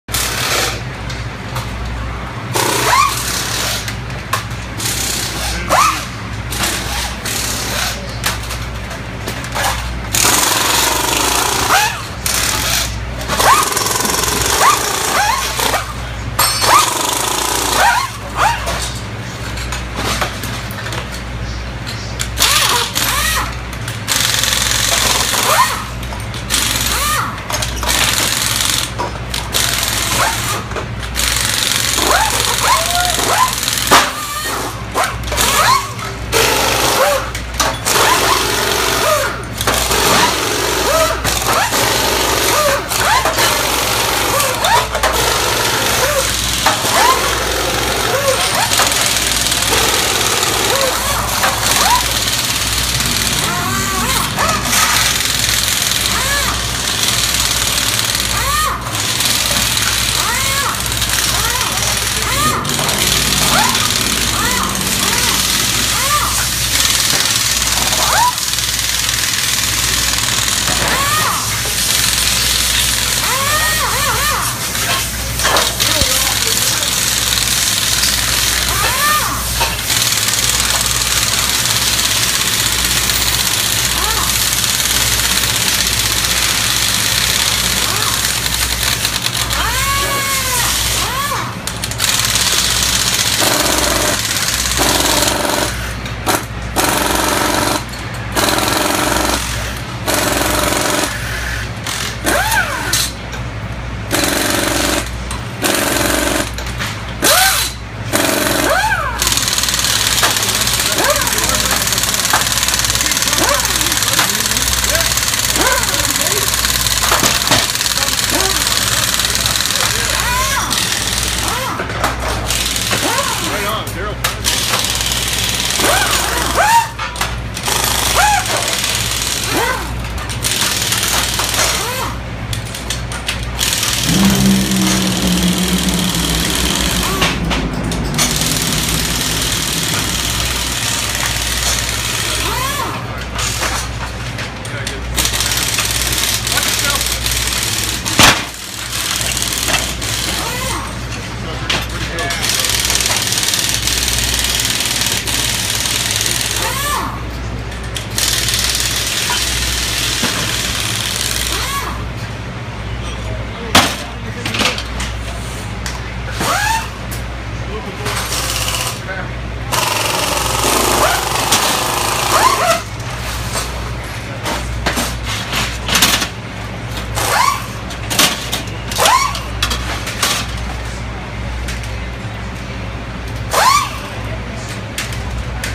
Lucas Oil Top Fuel Dragster Engine Disassembly - Part 2
Mopar Mile High Nationals 2015